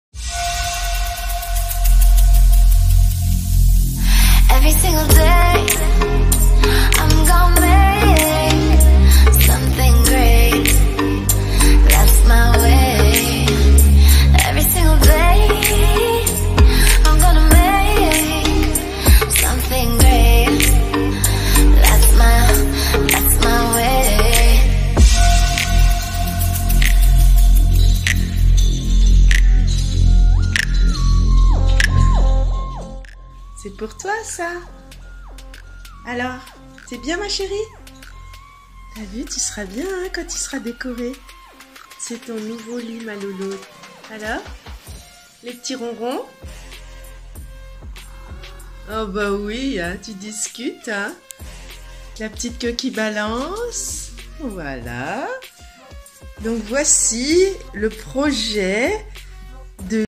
Un lit suspendu pour chat made in de Toulaba, c'est ici
Idéal pour votre matou, il s'accroche sur le radiateur. 1er avantage, votre chat sera bien au chaud l'hiver. 2e avantage, il est en hauteur comme il aime. 3e avantage, exercice pour sauter dans son lit… En tous les ça, ma Luna, elle ne le quitte plus, ELLE L'ADORE !!!